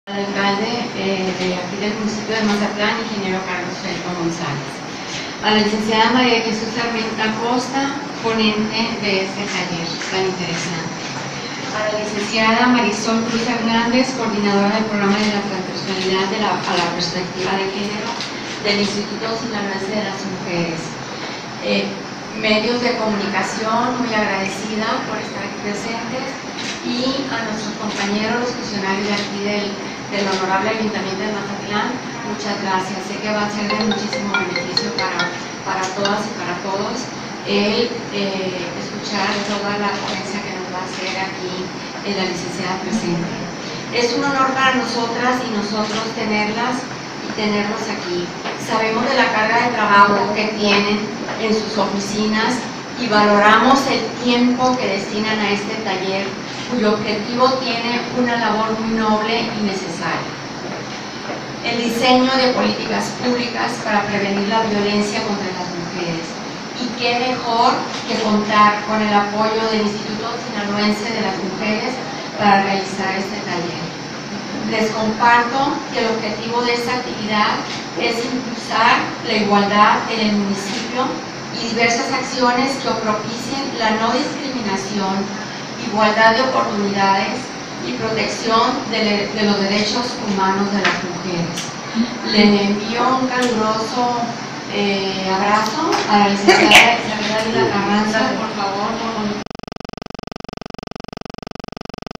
Taller Diseño
Inauguran  taller Diseño de las Políticas Públicas para Prevenir la Violencia contra las Mujeres, con el objetivo de impulsar la igualdad en el municipio, la no discriminación, igualdad de oportunidades y protección de los derechos humanos de las mujeres.